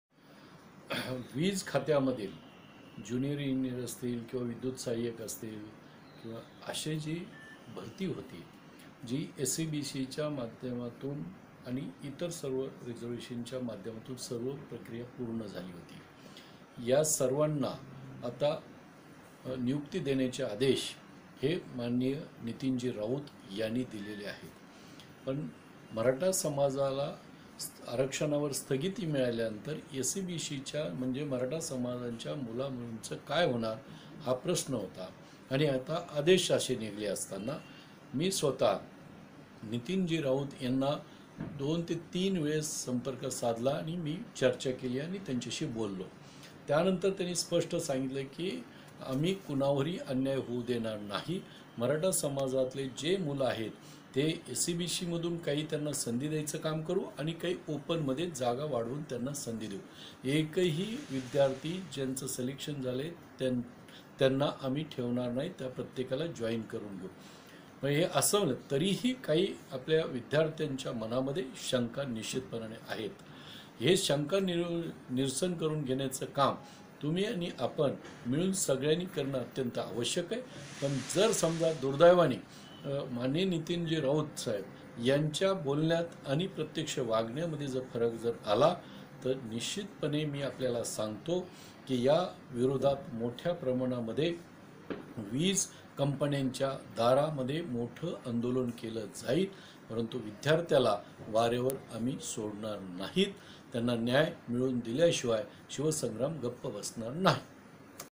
आश्वासन पाळा, अन्यथा आंदोलन..उर्जामंत्र्यांना विनायक मेटेंचा इशारा
मुंबई : महावितरणच्या भरती प्रक्रियेत कोणावर अन्याय होणार नाही, असा आश्वासन ऊर्जामंत्री नितीन राऊत यांनी दिलं असल्याचं मत शिवसंग्रामचे अध्यक्ष विनायक मेटे यांनी व्यक्त केलं. शिवाय उर्जामंत्र्यांनी दिलेलं आश्वासन पाळलं नाही तर आंदोलन करण्याचा इशाराही विनायक मेटे यांनी दिला आहे.